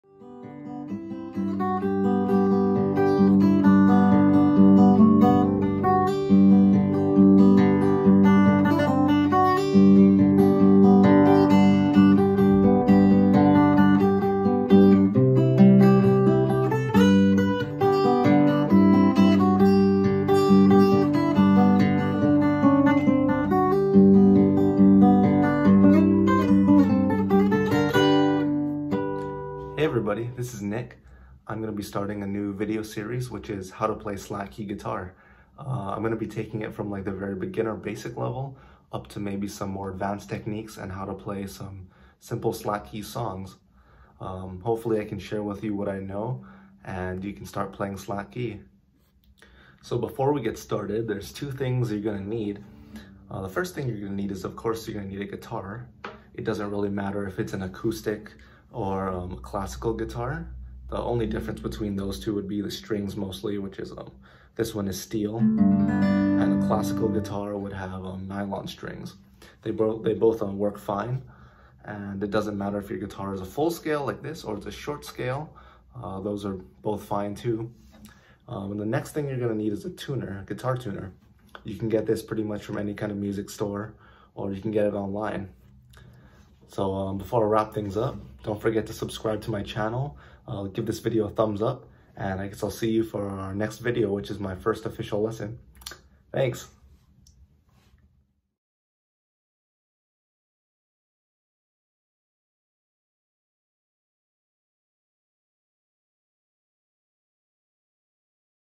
HOW TO PLAY Slack Key Guitar #1 “Introduction”
This is my introduction to my new slack key tutorial video series. Hopefully I can share some slack key guitar techniques and some of the basics.

Note: Guitar Tuner apps on your phone will work too.

(For ukulele players:
If you play ukulele, the same concepts can be applied. just disregard strings 5 and 6. Then de-tune the A string to a G.)